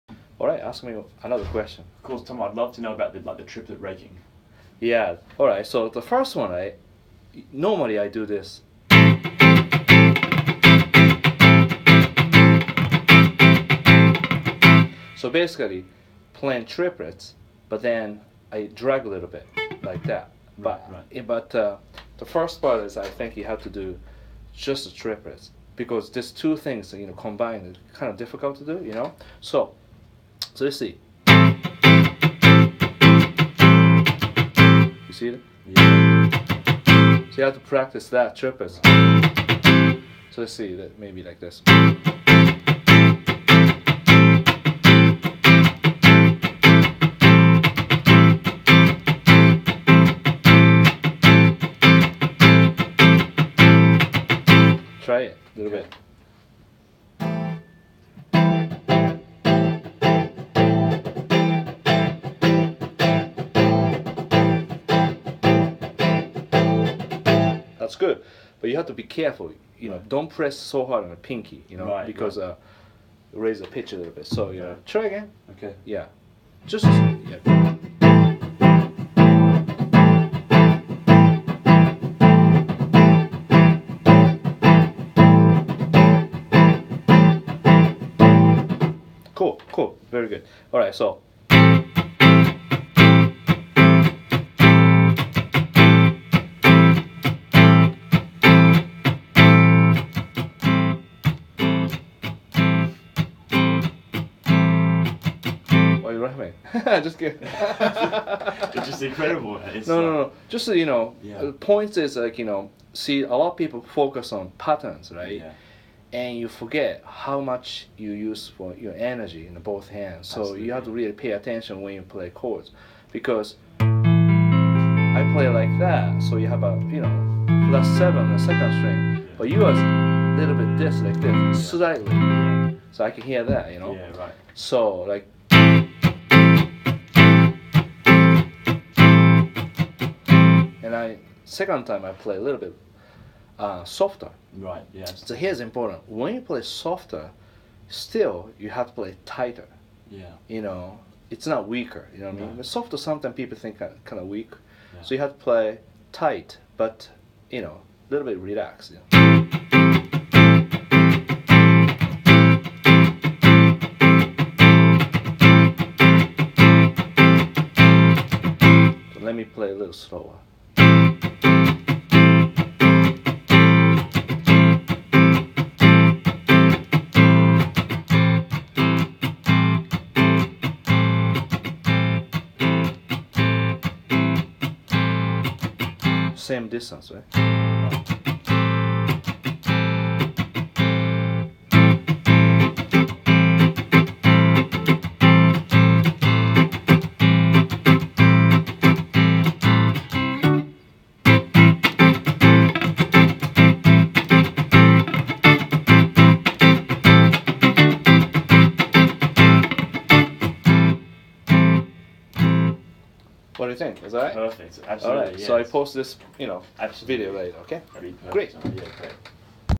Berklee Private Guitar Lesson / My Berklee student asked my triplet raking technique - Tomo Fujita
One of my students at Berklee asked me my triplet raking technique so I answered here. Hope you like this video.

Tomo